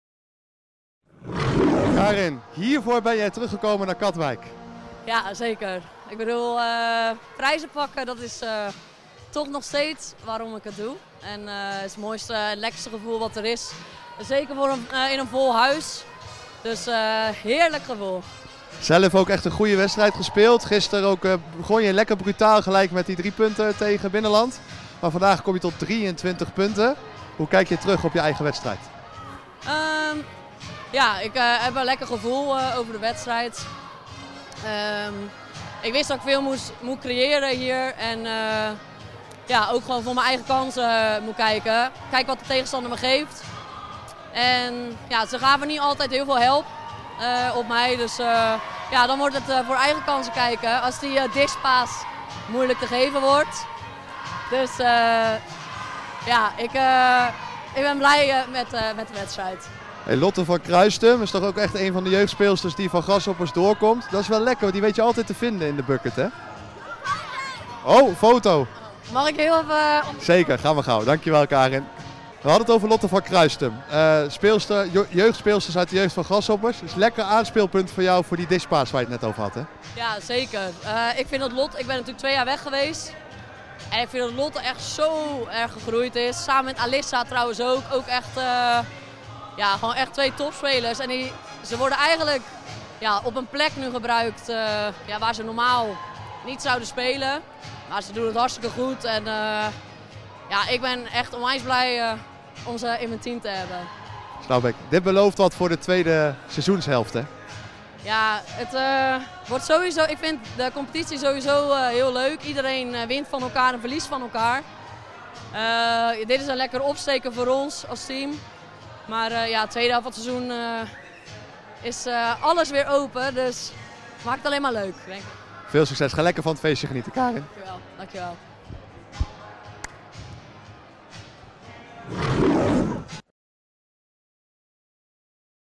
Karin Kuijt: "Heerlijk om zo in eigen huis deze prijs te pakken"
Na de winst op Binnenland en de finale tegen Den Helder mag Sportiff Grasshoppers een prijs toevoegen aan de prijzenkast. Ze winnen namelijk in eigen huis de Final4, de strijd om het kerstkampioenschap in de Women's Basketball Leauge. Na de wedstrijd sprak Steven Teunissen met Karin Kuijt, die een enorm belangrijke rol had in de finale. Even werd het interview onderbroken voor de belangrijke foto's, maar Karin blikt terug op een heerlijk weekend.